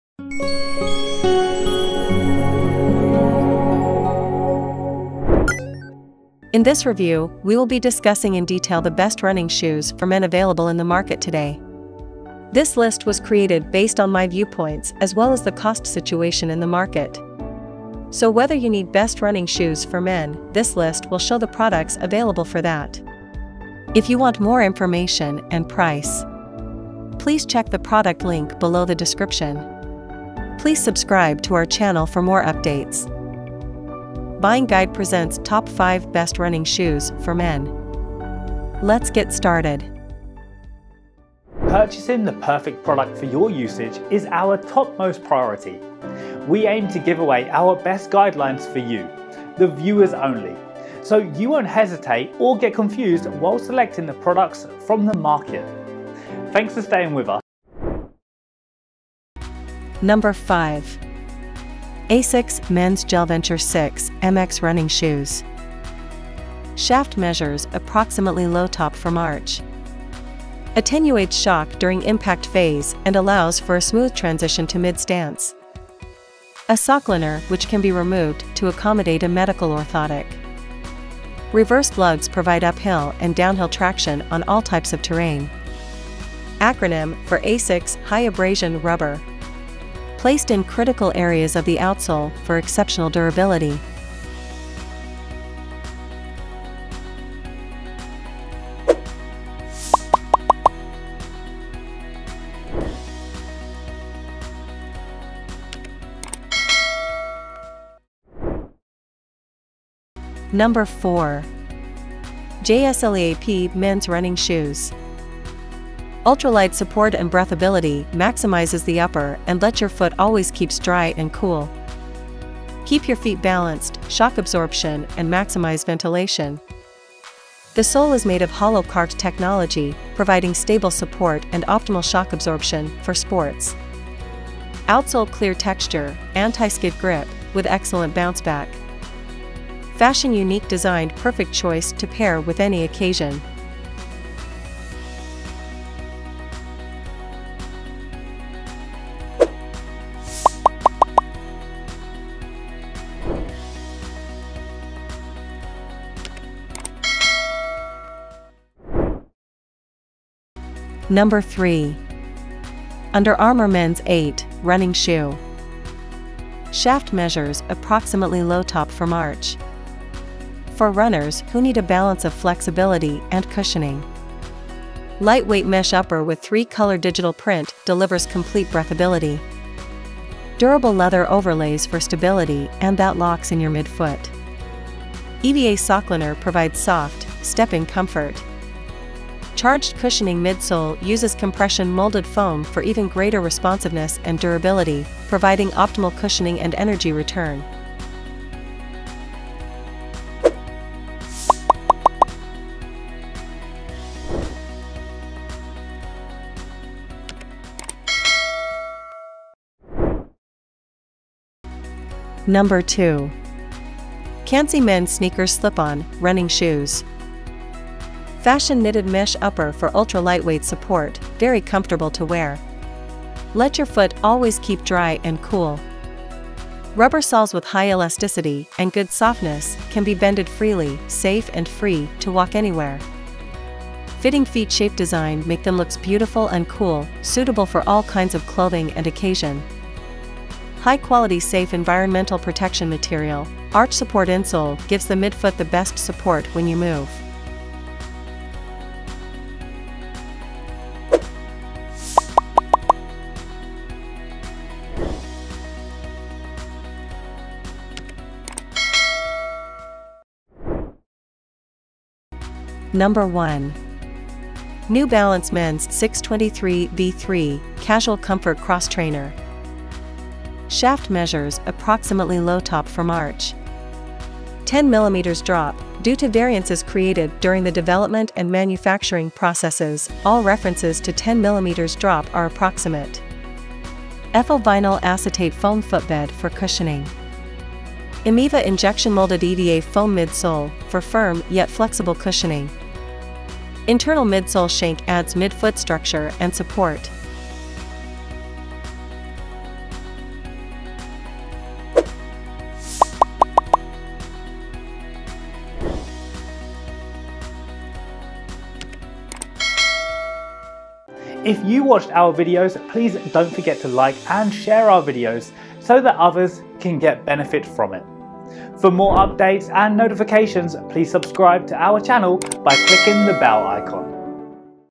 Best Running Shoes For Men in 2022 - Ensuring the Best Quality
In this video, you’ll know about the exciting features of Running Shoes For Men at the best price. So, we’ve chosen the Best Running Shoes For Men based on quality, features, performance, and price. Choose any of them that matches your considerations.

Best Sellers in 2022

00:00 - Introduction : Best Running Shoes For Men 2022

00:59 - 5. ASICS Men's Gel-Venture 6 MX Running Shoes

01:50 - 4. JSLEAP Mens Running Shoes

02:40 - 3. Under Armour Men's 8 Running Shoe

03:32 - 2. QANSI Mens Sneakers Slip-on Running Shoes

04:22 - 1. New Balance Men's 623 V3 Casual Comfort Cross Trainer

Best Running Shoes For Men in 2022,Top Running Shoes For Men in 2022,Best Running Shoes For Men Reviews